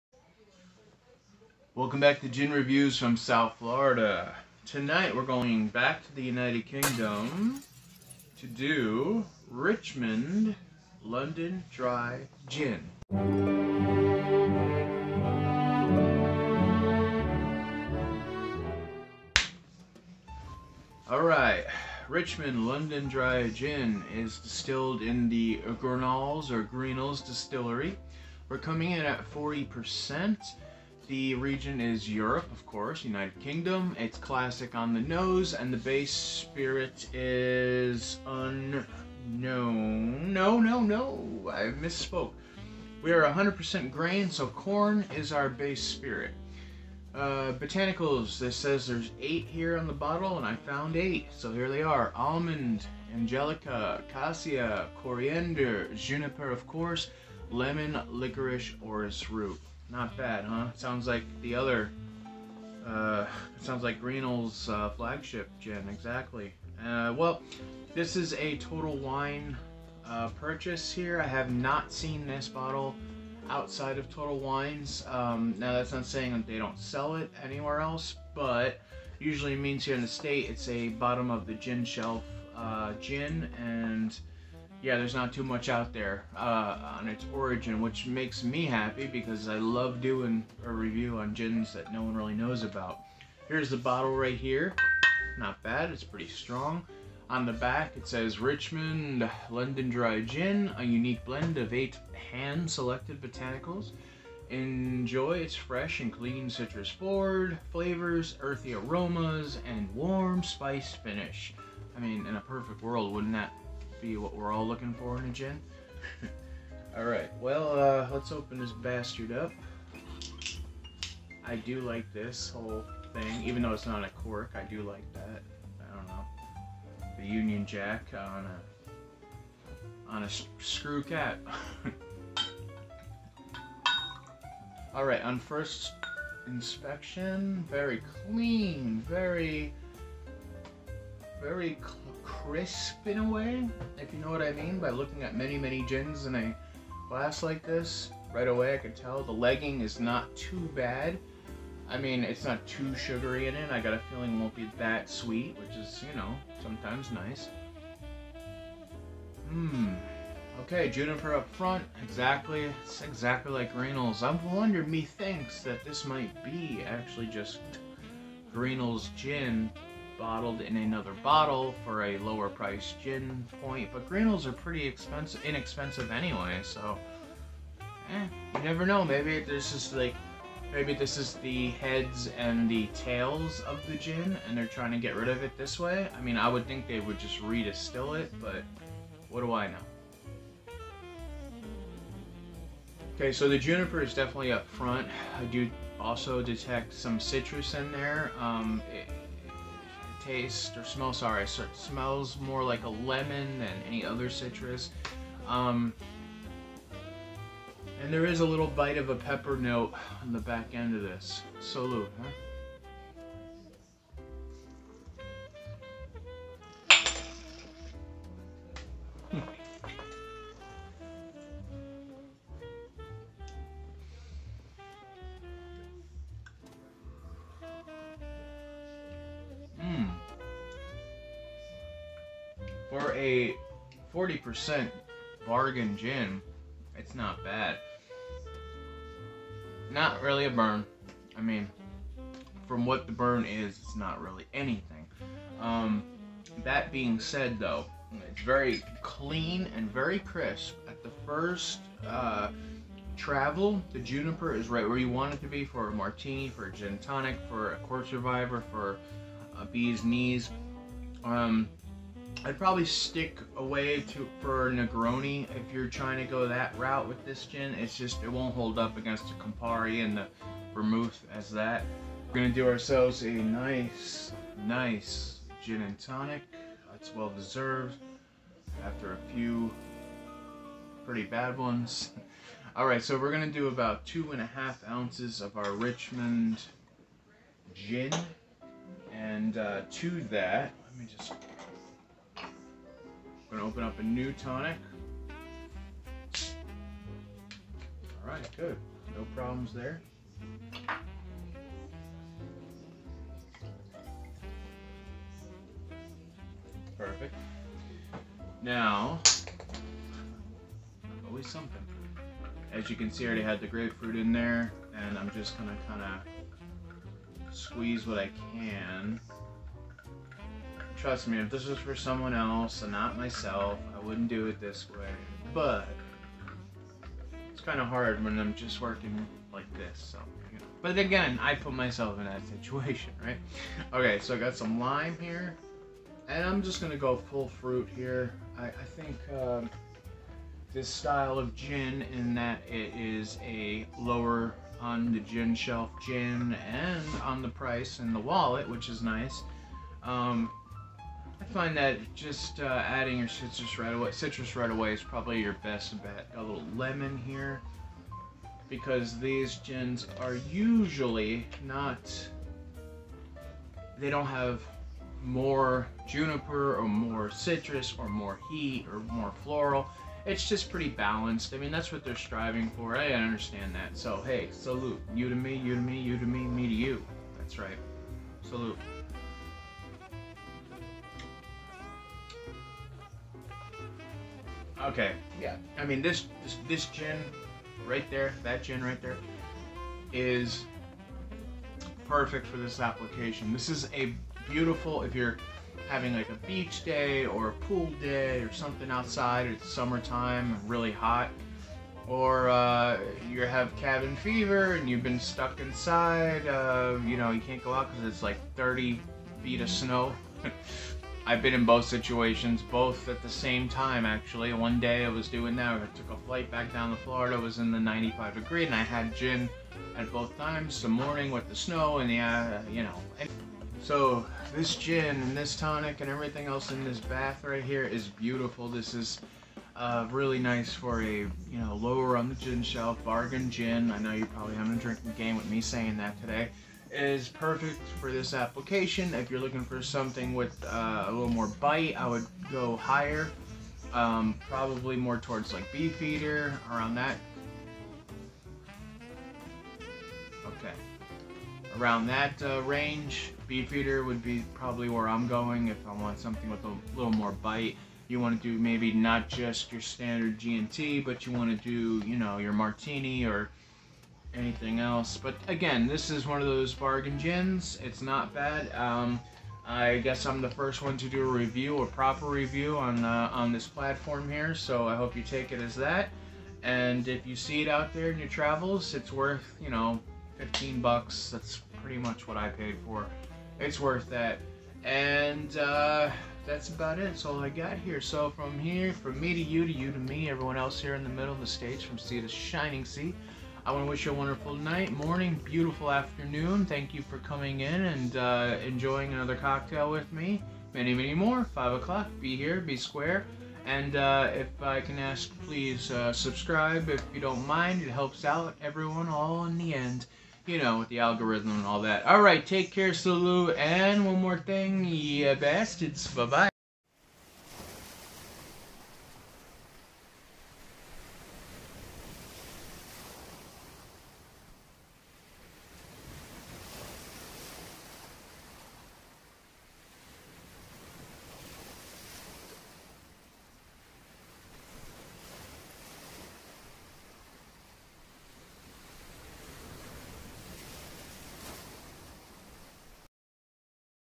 Will this Gin "Richmond" be a Yeh or Neh??!
another bottom of the Gin shelf Gin being reviewed on the only channel that will review 7 days a week!!😎